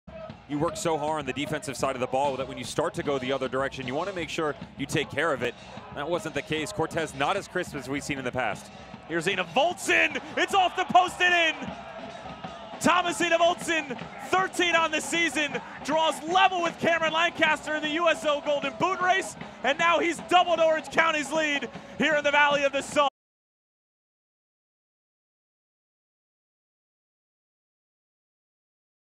Week 22: Orange County SC vs. Phoenix Rising FC Goal by Thomas Enevoldsen
🥅 GOAL 🥅 @orangecountysc goal by forward Thomas Enevoldsen PHXvOC 🍊⚽️